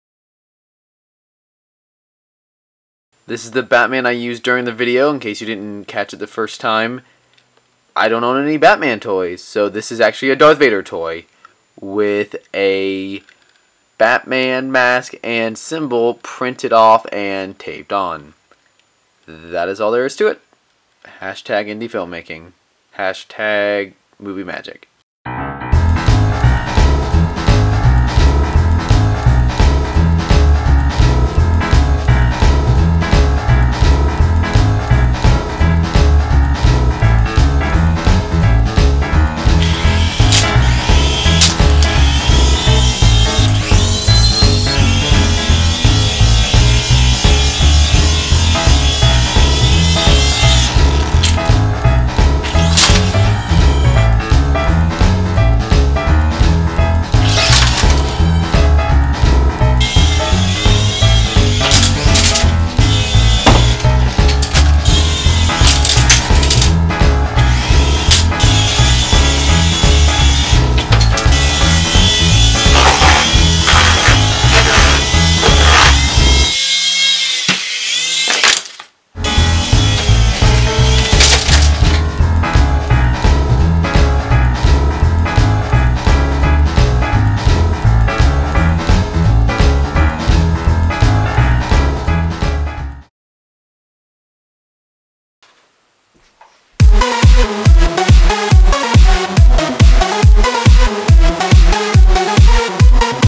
Batman v. Superman On a Budget - Outtakes
The title says it all. I made a video called "Batman v. Superman On a Budget," and these are the outtakes.